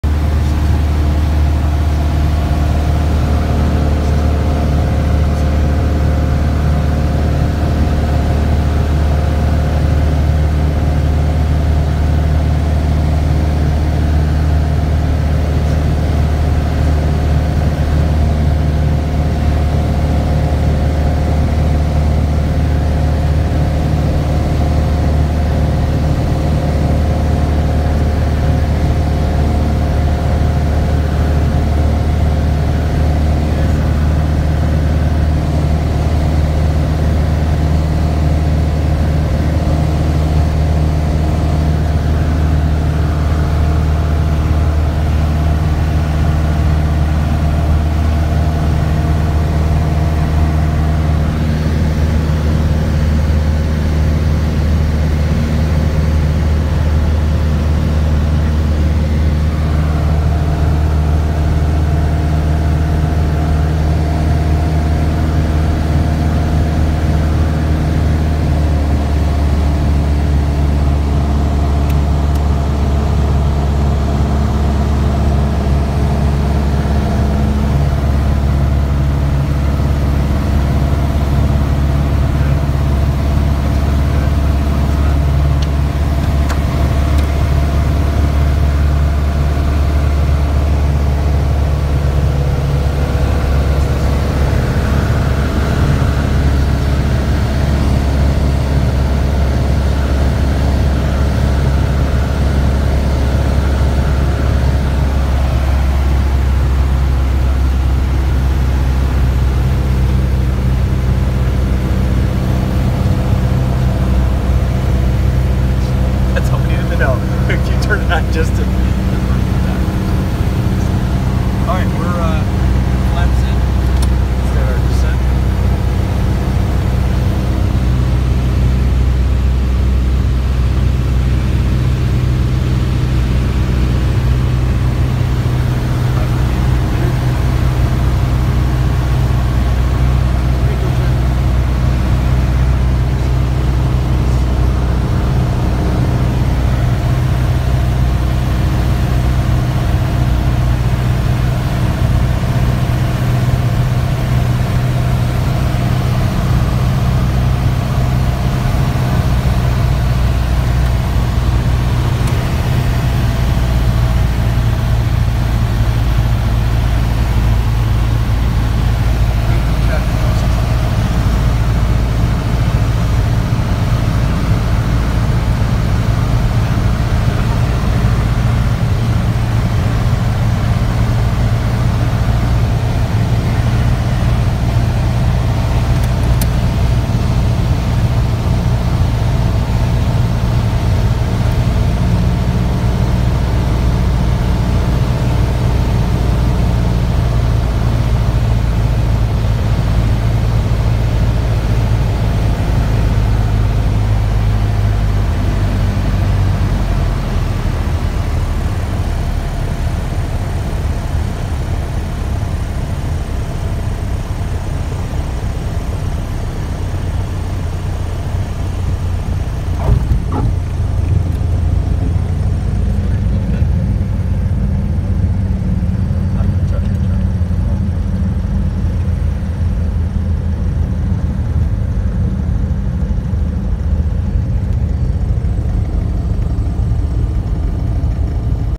Flying to the Appalachian mountain region to Airdrop supplies
Our flight into the flood damaged area of western North Carolina and eastern Tennessee took us into the mountainous part of the east coast.